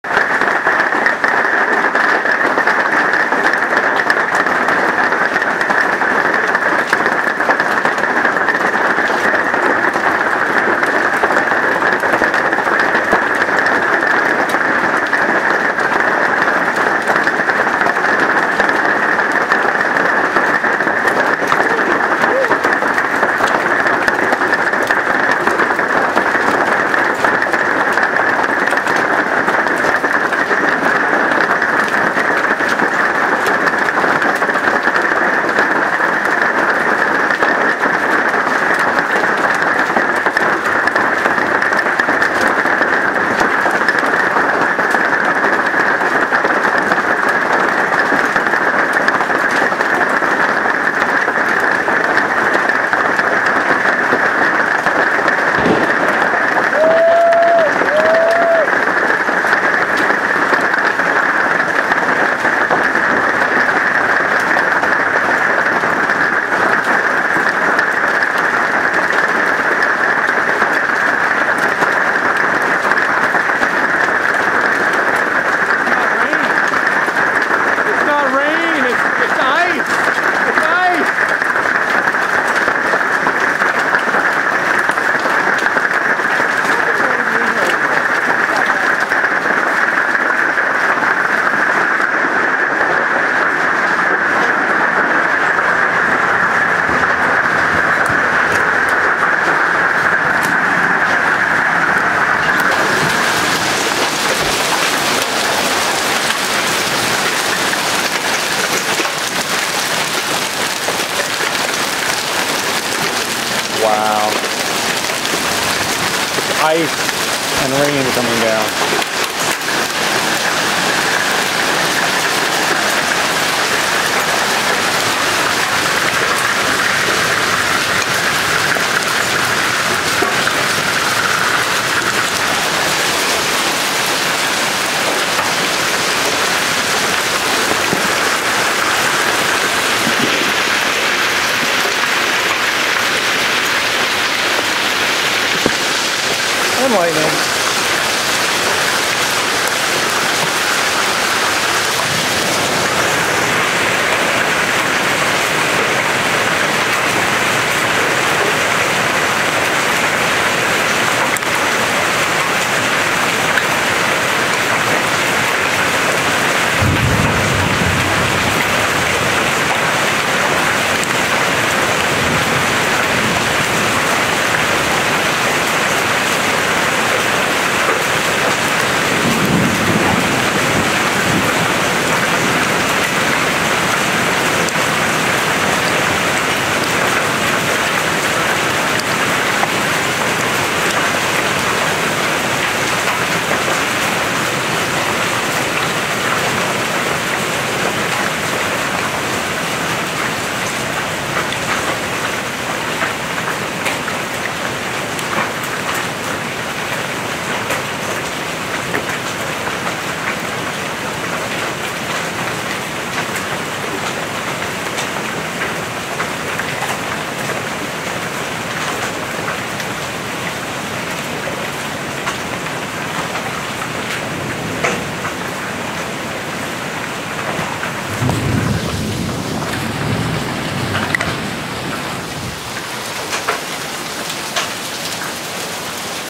Hail Hitting in Gainesville Florida 03/14/2012
Video of the Hail hitting my house on 03/14/2012 at 7:25 pm